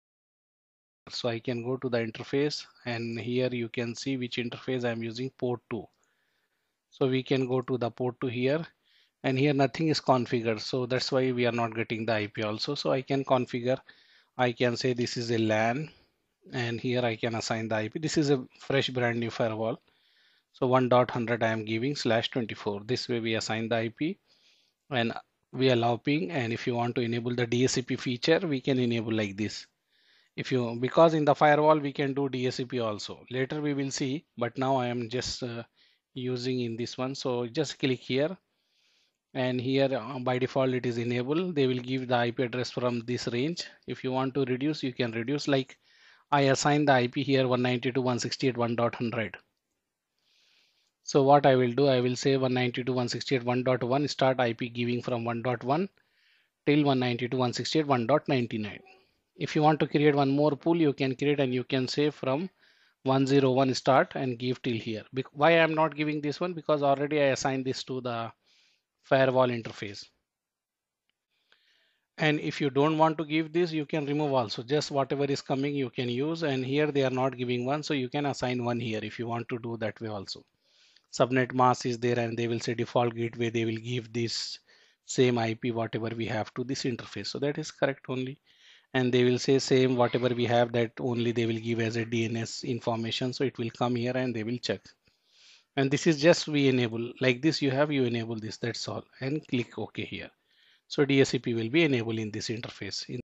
2.How to Assign IP address to Fortigate Firewall Interface
How to Assign IP address to Fortigate Firewall Interface

Hello Everyone,

In this video we are going to discuss How to Assign IP address to Fortigate Firewall Interface?
Thanks.

About us:
Thanks